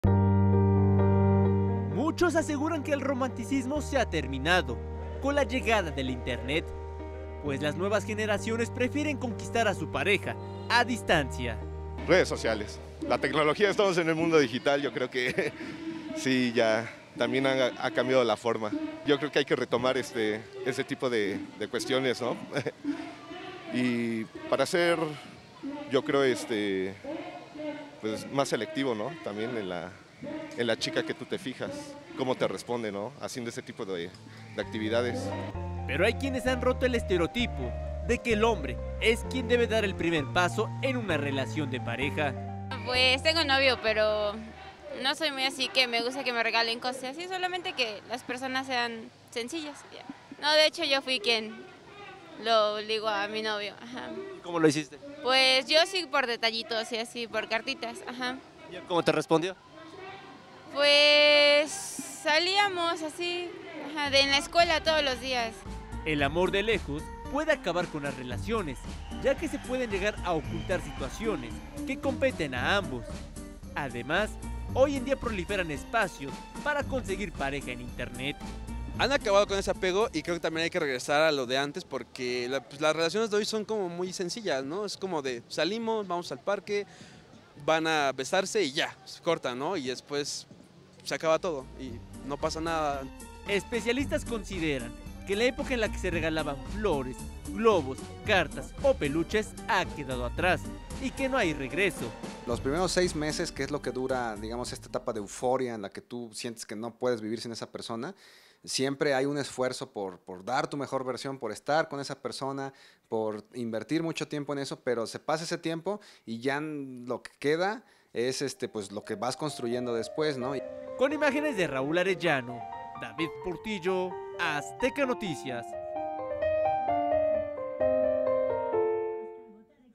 Amor por internet
El mundo digital a transformado las relaciones amorosas desde la forma de conquistar a tu pareja, los detalles y la forma en frstejar este 14 de febrero.